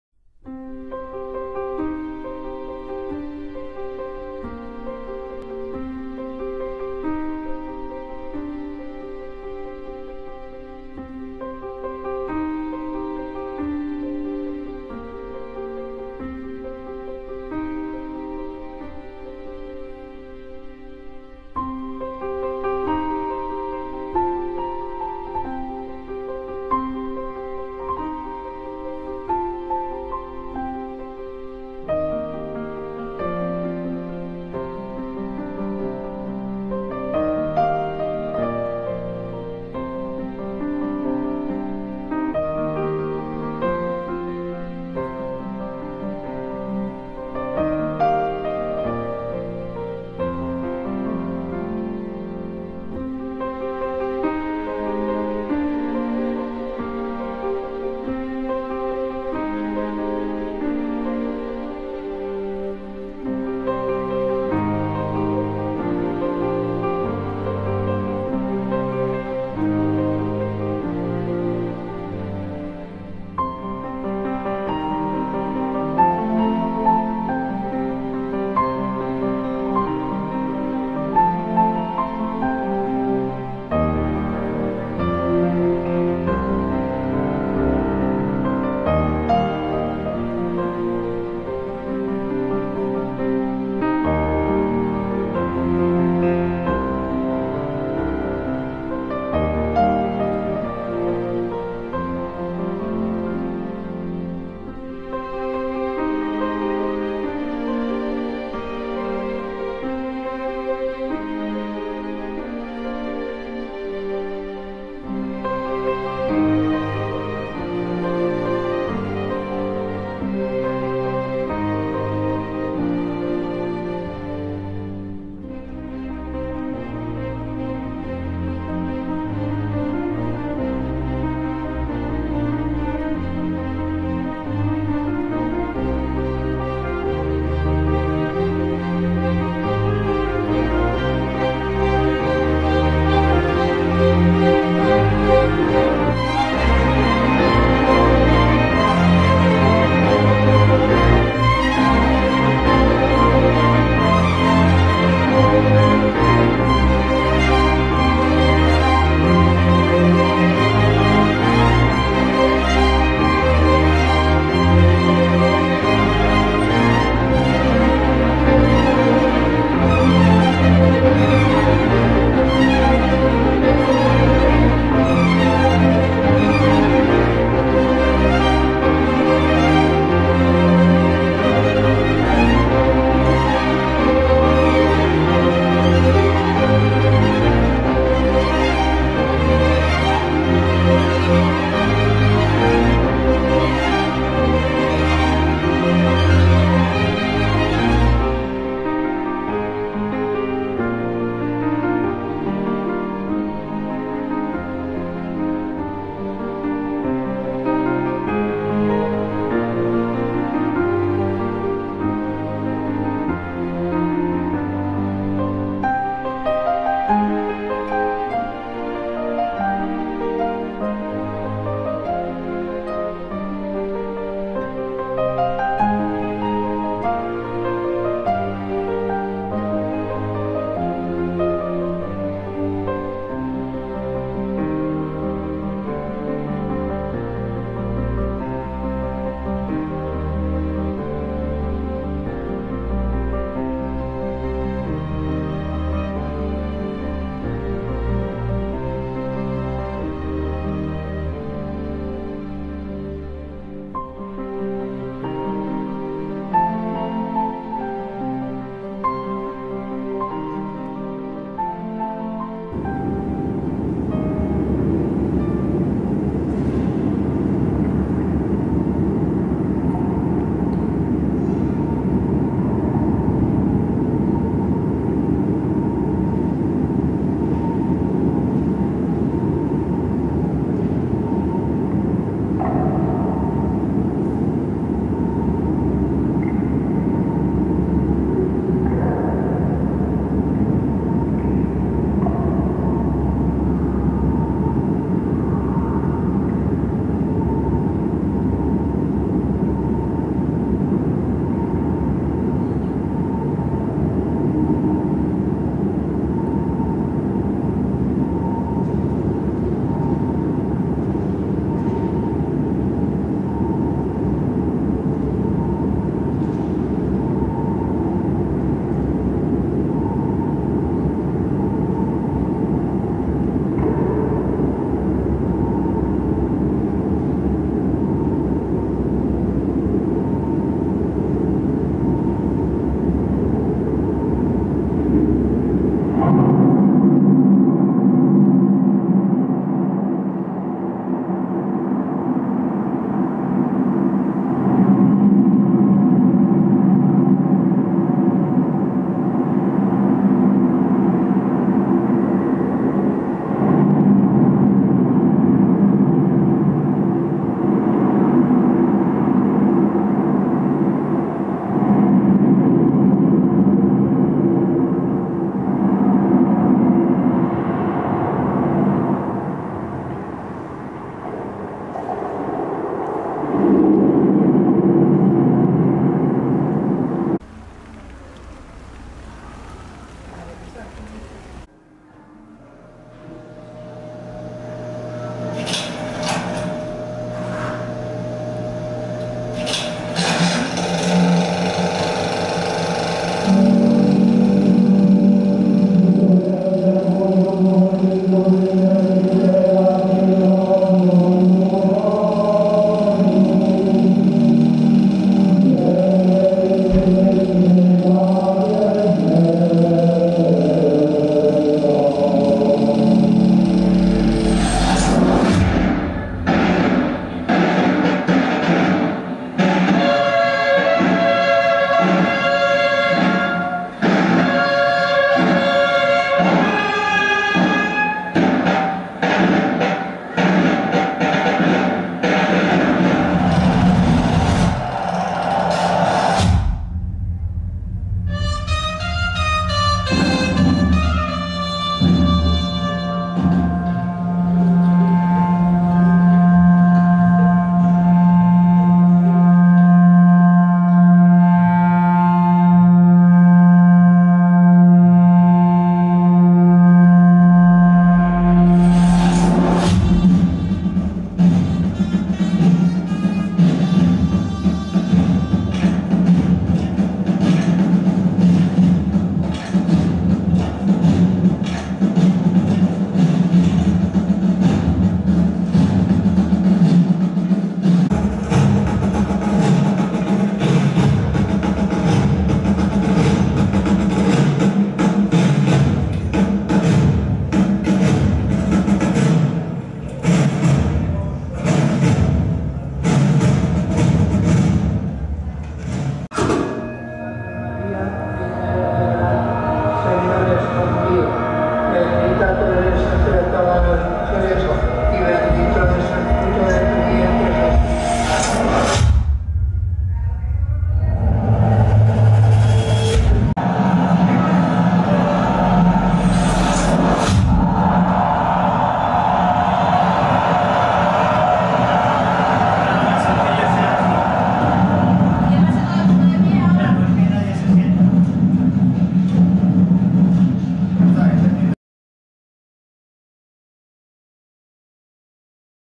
Exposición “Vía Mística” de Bill Viola
La muestra ‘Bill Viola: Vía Mística’ es una nueva iniciativa expositiva y cultural de alcance nacional e internacional, promovida por el Gobierno regional a través de la Fundación Impulsa Castilla-La Mancha, que cuenta con la colaboración del Ayuntamiento de Cuenca, la Fundación Juan March, la Junta de Cofradías de Cuenca, la escuela Cruz Novillo y el Colectivo Lamosa.